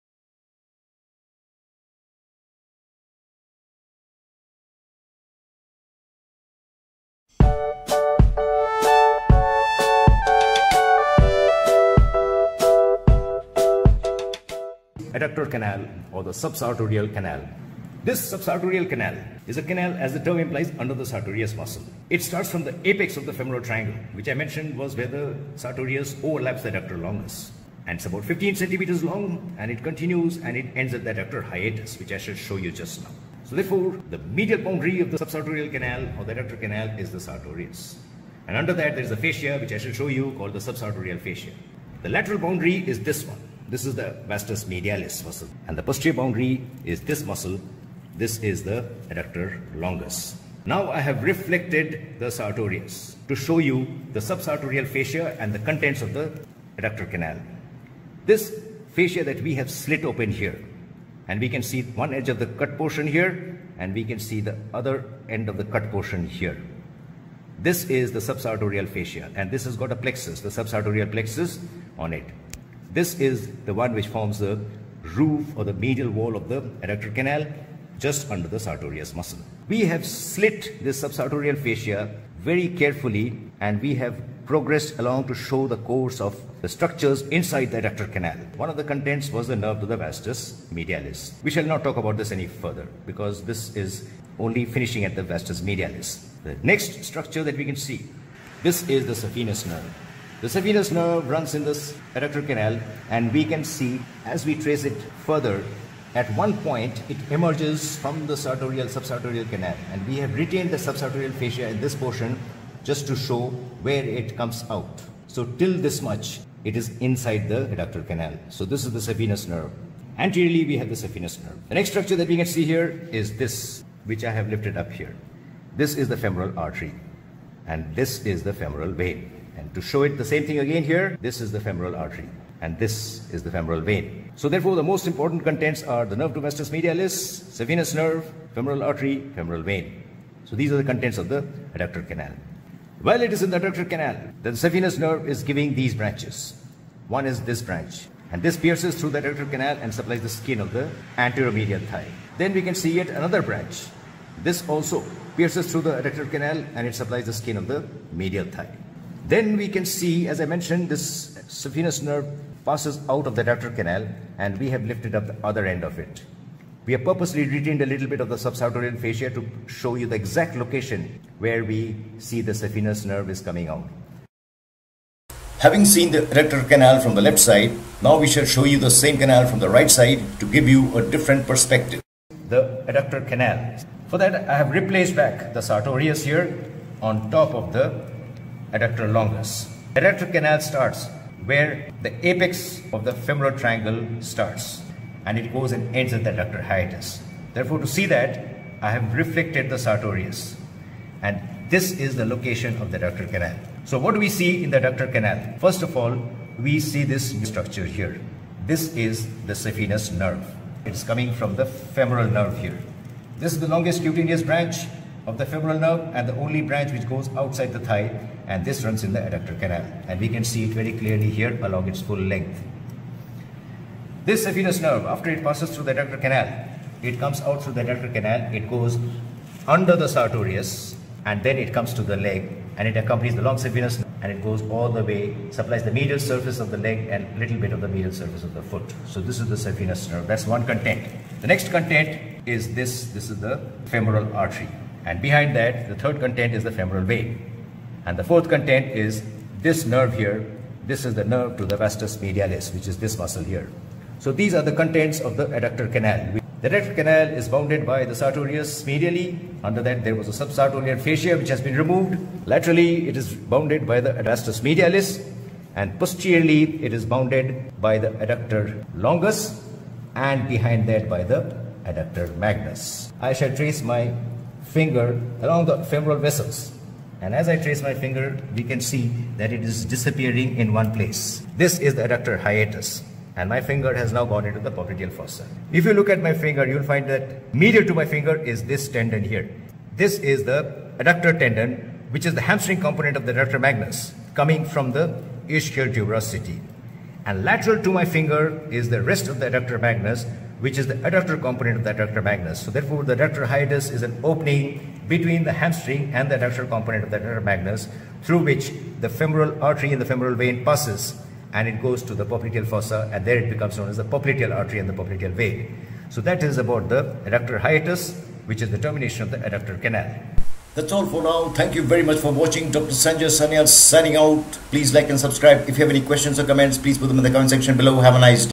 SUBSARTORIAL CANAL Saphenous Nerve Femoral Vessels and Clinical Aspects – Sanjoy Sanyal – Proceum
It shows the surgical anatomy of the Femoral Triangle and Adductor Canal. The specimen was harvested from a cadaver.
It also shows the Adductor Muscles.
Cameraperson was Kendall Cumberbatch, using my Samsung Galaxy S8 with 8/12 MP rear and forward viewing cameras. Hope you will not disappoint him.